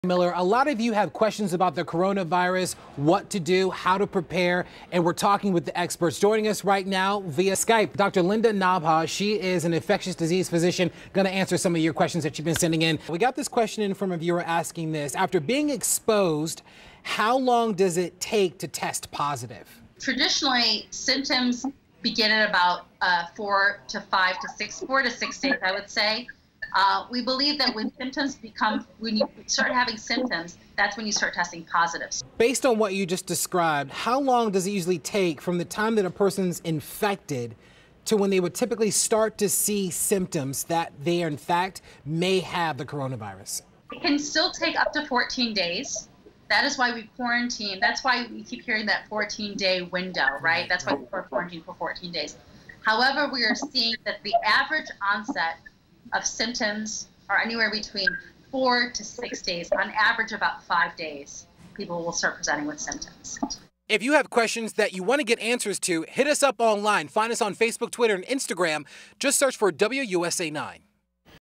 How long after being exposed does it take to test positive?
Infectious disease specialist Dr. Linda Nabha joins us again to answer more questions about Coronavirus